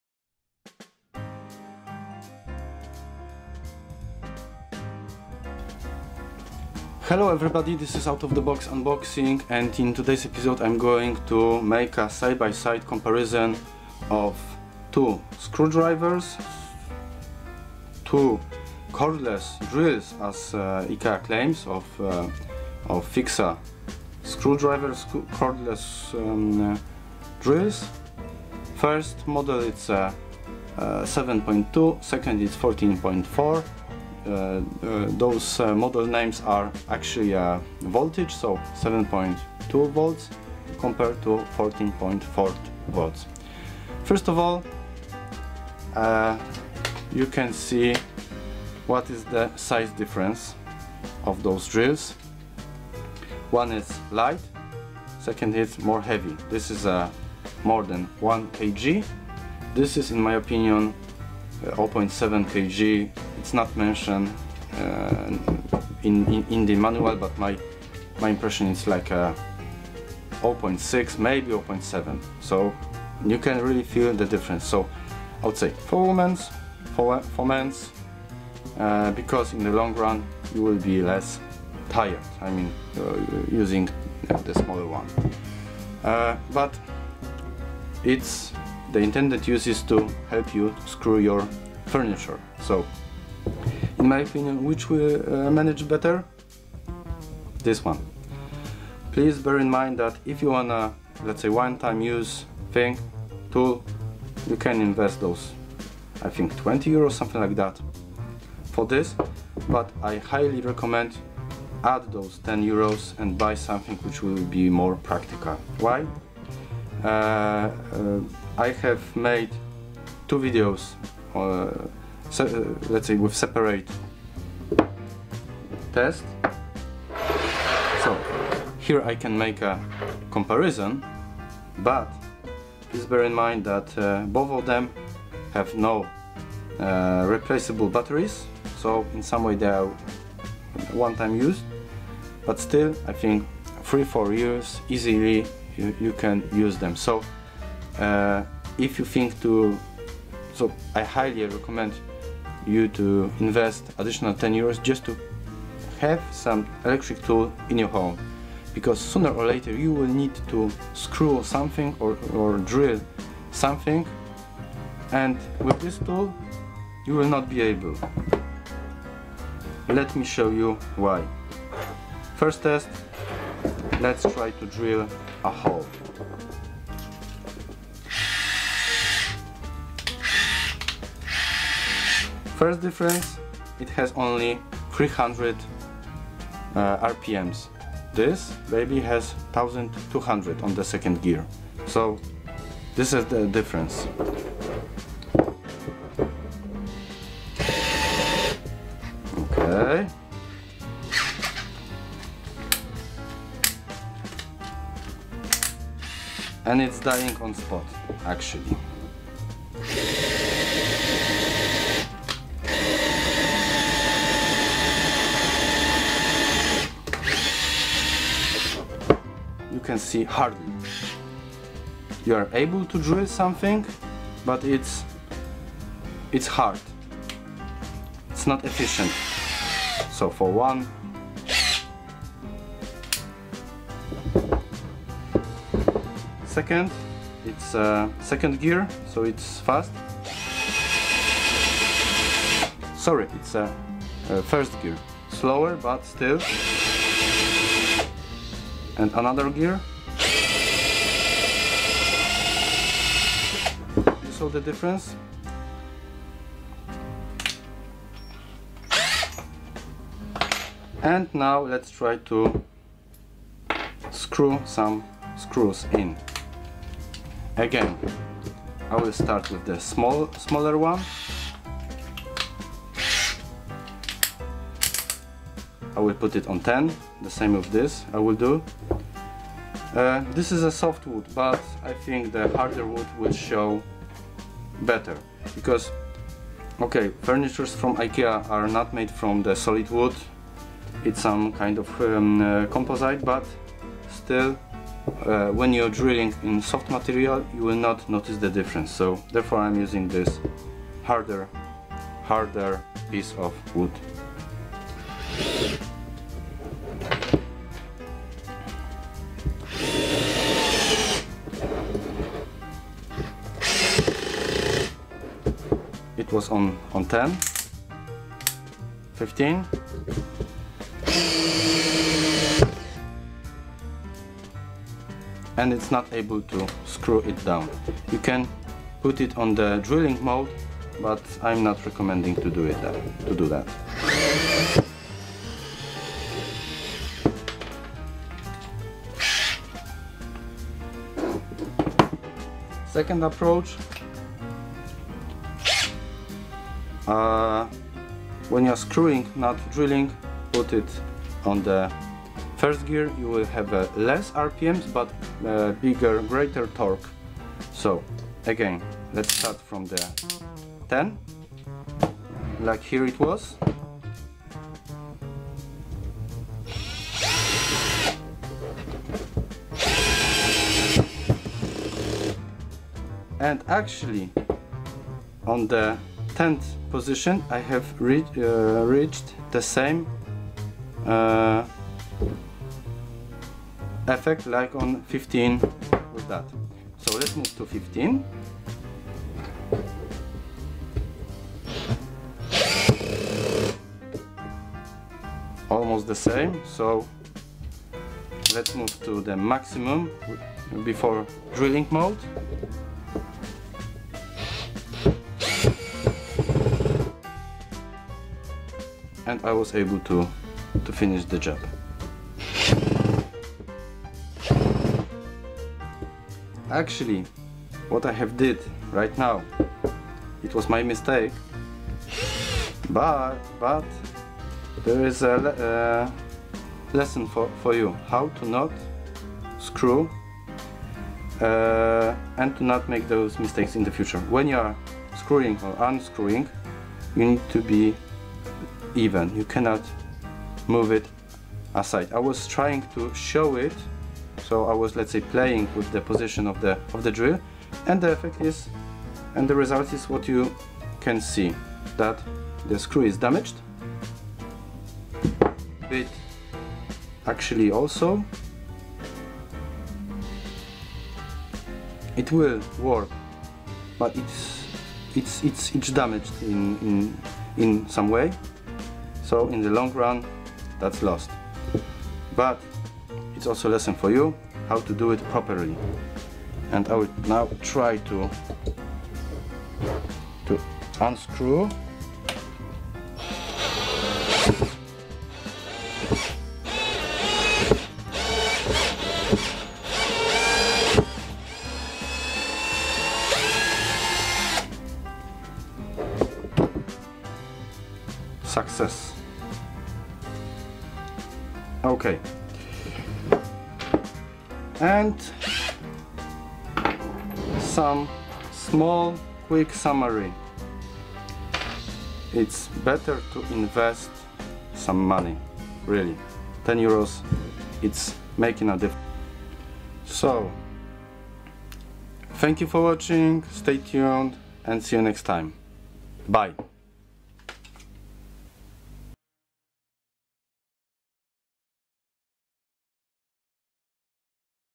FIXA Screwdriver / drill 7.2V VS 14.4V side by side comparision (IKEA li-ion, 7.2 V 14.4 V)
7.2V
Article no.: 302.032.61
1-speed cordless screwdriver/drill for general screwdriving or simple drilling work.
15 torque settings for screwdriving allow you to adjust the amount of torque to suit different tasks.
Built-in lithium-ion battery with low self-discharge rate so the power tool is ready to use whenever you need it.
Electronic Cell Protection (ECP) prevents the battery from deep discharge, overloading and overheating, which could damage the battery.
Easy to grip handle of soft synthetic rubber.
Rotational direction switch for rotation right or left.
Max. rpm (no-load speeds): 0-400/minute.
Max. torque: 5 Nm.
Holder for screwdriver bits: 6 mm.
Chuck: 10 mm, keyless, quick-fit quick-release chuck.
Built-in rechargeable lithium-ion battery 7.2V/1.5 Ah.
The battery must be fully charged before first time use.
Battery charger included.
The battery can be charged up to 500 times.
10 screwdriver bits, 2 drills of dia. 3 and 5 mm and 1 bit holder included.

14.4V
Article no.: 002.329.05
2-speed cordless screwdriver/drill, one gear with low speeds for powerful screwdriving and one gear with high speeds for drilling.
19 torque settings for screwdriving allow you to adjust the amount of torque to suit many different tasks.
Max. rpm (no-lead speed): position I: 0-350/minute, position II: 0-1200/minute.
Max. torque: 16 Nm.
Built-in rechargeable lithium-ion battery 14.4V/1.3 Ah.
Battery charger included.
10 screwdriver bits, 2 drills of dia. 3 and 5 mm and 1 bit holder included.